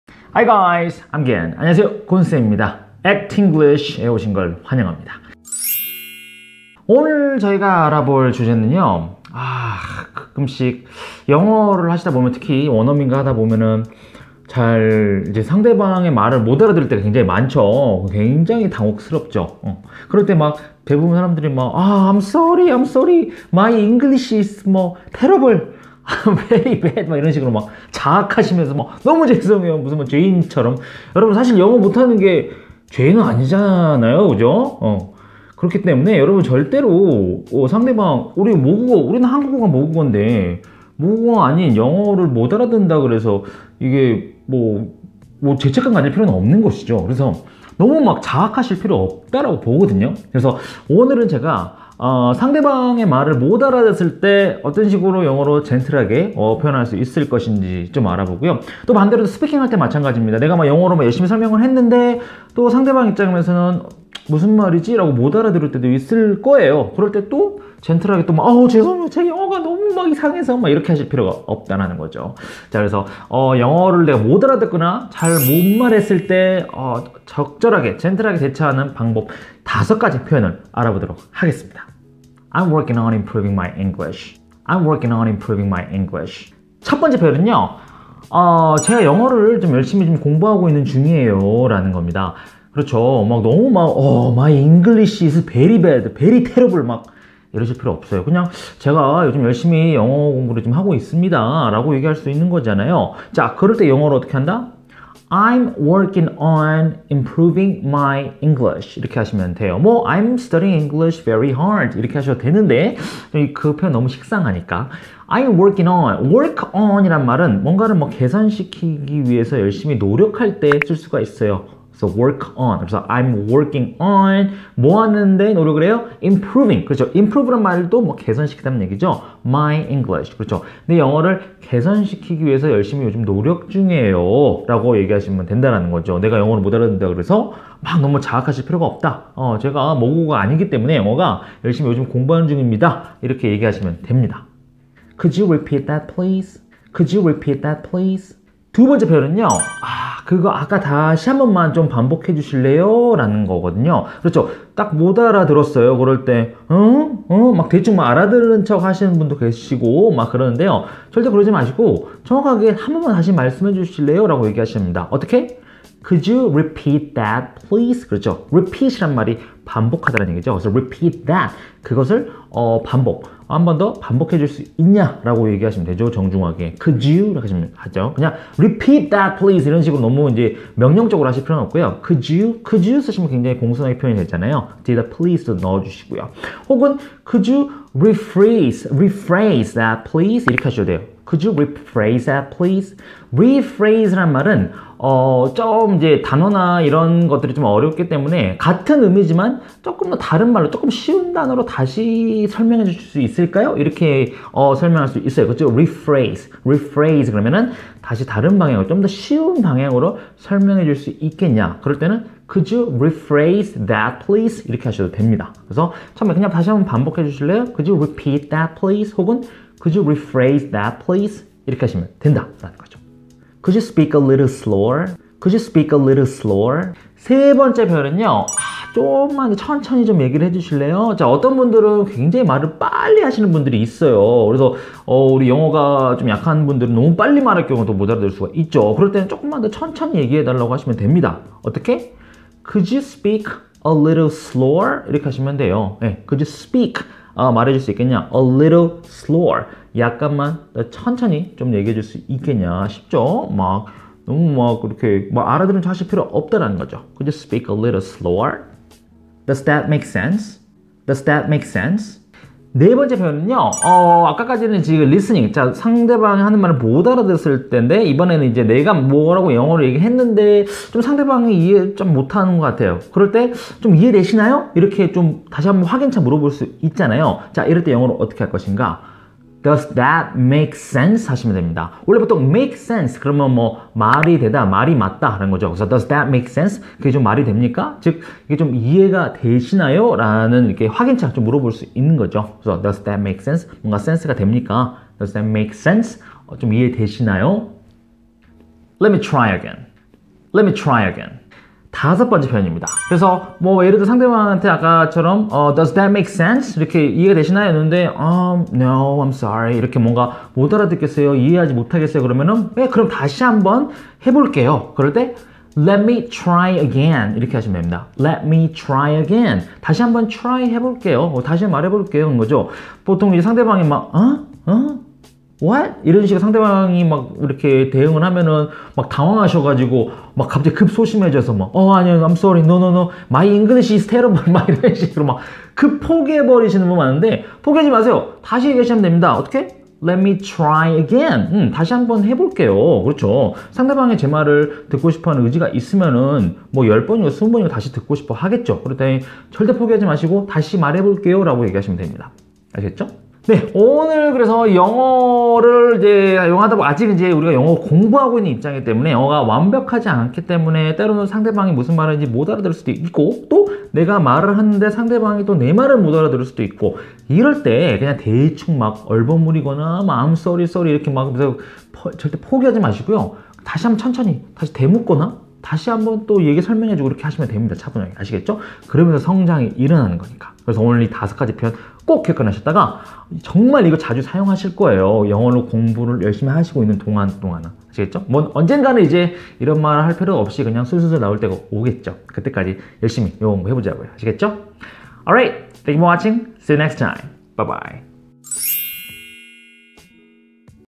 [Day6] 원어민이 하는 말이 이해가 안될 때 알아야 할 표현 5가지
6. English mistake

1. 제 영어를 늘려가는 중 이에요.
I'm working on improving my English.
I'm dedicating some time each day to working on improving my English.
I've been attending an online English class to work on improving my English.
My goal for this year is to keep working on improving my English.

2. 다시 말씀해주실래요?
Could you repeat(or rephrase) that, please?
Could you repeat what you just said, please?
I didn't quite understand that, could you rephrase it for me?
I'm sorry, could you repeat your question again?

3. 좀 천천히 말해주실래요?
Could you speak a little slower?
Could you please speak a little slower so that I can understand you better?
I'm having a hard time following you, could you speak a little slower?
Could you slow down a bit, please?

4. 이해가 되시나요?
Does that make sense?
I'm not sure I understand what you mean, does that make sense?
I'm having trouble following your logic, does that make sense?
So, the plan is to start early and finish late, but how will we manage the workload? Does that make sense?

5. 다시 해볼게요.
Let me try again.
I don't think I explained myself well enough, let me try again.
I think I misunderstood your question, let me try again.
That didn't come out right, let me try again.